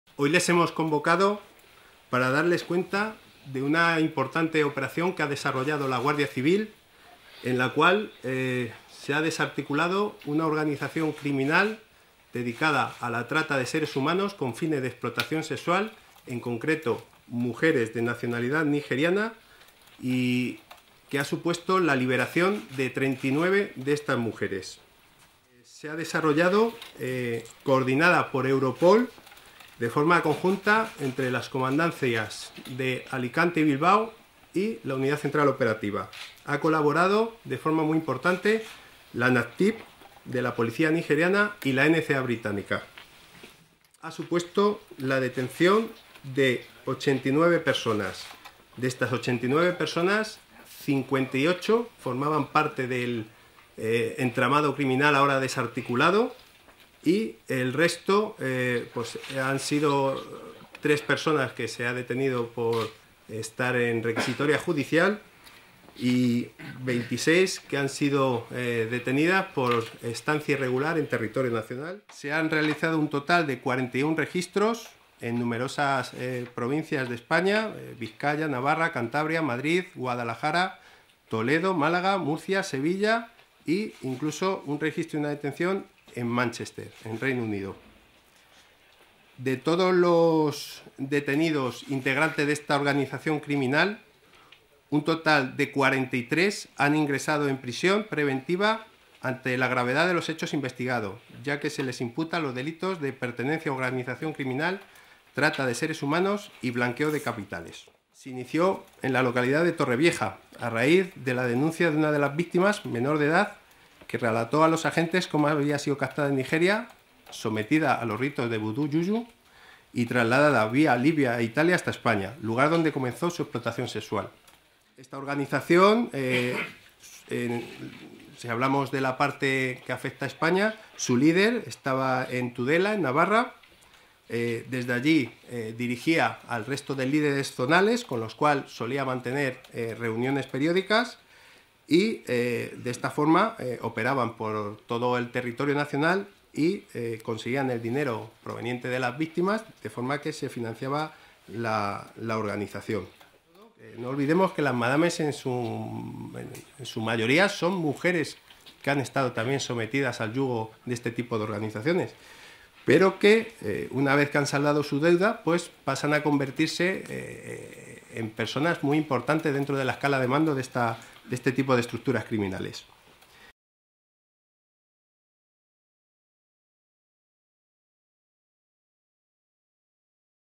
Rueda de Prensa Operación Nanga-Parbat. Operaciones Guardia Civil. UCO
La Guardia Civil, en colaboración con la  NAPTIP de Nigeria (National Agency for the Prohibition of Trafficking in Persons) y  la NCA Británica, en una investigación coordinada por EUROPOL, ha desarrollado la operación NANGA-PARBAT en las provincias de Vizcaya, Navarra, Cantabria, Madrid, Guadalajara, Toledo, Alicante, Málaga, Murcia,  Sevilla, Almería y Barcelona, contra la Trata de Seres Humanos con fines de explotación sexual, desmantelando la infraestructura en España de la organización criminal nigeriana relacionada con la  EIYE CONFRATERNITY (La Confraternidad).

En la  operación han sido detenidas 89 personas, de las que a 58 se les imputan delitos de Organización Criminal, Trata de Seres Humanos y Blanqueo de Capitales. Han sido practicados 41 registros en las provincias de Vizcaya, Navarra, Cantabria, Madrid, Guadalajara, Toledo, Sevilla, Málaga, Murcia y Manchester (Reino Unido). De estos 58 detenidos, 43 han ingresado en prisión preventiva ante la gravedad de los hechos investigados.
Han sido liberadas 39 mujeres de nacionalidad nigeriana que estaban siendo explotadas sexualmente, 18 de las cuales relataron a los agentes las coacciones por el rito vudú al que eran sometidas desde su país de origen. En la localidad de Cuevas de Almanzora (Almería), las mujeres eran explotadas en el interior de casas-cueva, viviendo en unas condiciones de extrema precariedad.
La operación se inició en Torrevieja (Alicante) a raíz de la denuncia de una de las víctimas, menor de edad, que relató a los agentes como había sido captada en Nigeria y tras someterla en su país de origen a ritos de  Vudú-Jujú, fue trasladada, vía Libia e Italia, hasta  España, lugar donde comenzó su explotación sexual.
Durante la investigación los agentes pudieron comprobar cómo la organización tenía bajo su control a numerosas mujeres ejerciendo la prostitución en condiciones inhumanas para saldar la deuda adquirida con la organización que oscilaba entorno a los 30.000 euros.
Esta deuda corresponde a los gastos acumulados por el traslado a través de redes de inmigración irregular desde Nigeria hasta Europa, a través de la ruta Libia-Italia, de forma que todas estas mujeres, muy jóvenes e incluso menores de edad, eran tratadas como "mercancía" en un periplo que las llevaba desde Nigeria a Libia, recorriendo más de 4200 km por el continente africano. De las costas de Libia, vía marítima llegaban a Lampedusa (Italia), donde una vez en los campos de inmigrantes de dicha isla, eran recogidas por personas de la propia organización que las trasladaba a varios países europeos, entre ellos España. Llama la atención como este tipo de organizaciones criminales se sirve de la presencia activa en el panorama internacional de la delincuencia organizada en forma de redes de tráfico ilegal de inmigrantes.
Las mujeres eran controladas por las “madames”, de la misma nacionalidad, la mayoría reclutadas tras haber sido anteriormente explotadas en las mismas condiciones.
Estas  ”madames” cobraban a las víctimas para saldar su propia deuda y se aseguraban de enviar a los líderes su parte de las ganancias, por lo que las deudas tardan varios años en saldarse.

Uno de los miembros más importantes de la Organización es un conocido DJ en Nigeria, que fue detenido cuando regresaba a España procedente de dicho país africano, donde acababa de grabar un videoclip musical. Su función principal, dentro de la organización, era la de transportar a las víctimas, una vez que llegaban a Europa, de Italia hasta España y organizar la explotación sexual de las mismas en varias provincias españolas.
Investigación patrimonial y Red Hawala
El fin último de la explotación sexual de las mujeres no es otro que el enriquecimiento de la Organización. La investigación patrimonial realizada a sus líderes demuestra la existencia de toda una red entre los integrantes de la Confraternidad y las diferentes "madames" que hacían acopio del dinero de las víctimas que controlaban. Una gran parte de todo este dinero se remitía a Nigeria.
La Guardia Civil ha trabajado en colaboración con la  NAPTIP de Nigeria (National Agency for the Prohibition of Trafficking in Persons), junto con la NCA Británica, en una operación coordinada por EUROPOL.
Es la primera vez en España que se produce este tipo de colaboración operativa sobre el terreno con la presencia de policías desplazados expresamente desde Nigeria en los registros, análisis de documentación, asistencia a las víctimas y consultas en bases de datos, lo que ha permitido a los investigadores poder conocer la forma de captación de las mujeres en Nigeria y sus rutas de traslados a Europa, así como actuar de forma simultánea y coordinada en Nigeria con la información que aquí se iba obteniendo de las distintas actuaciones realizadas.
 También se ha contado con la colaboración de la NCA Británica, deteniendo en la localidad de Manchester a una de las "madames" que había captado y  trasladado a varias víctimas desde Nigeria hasta España.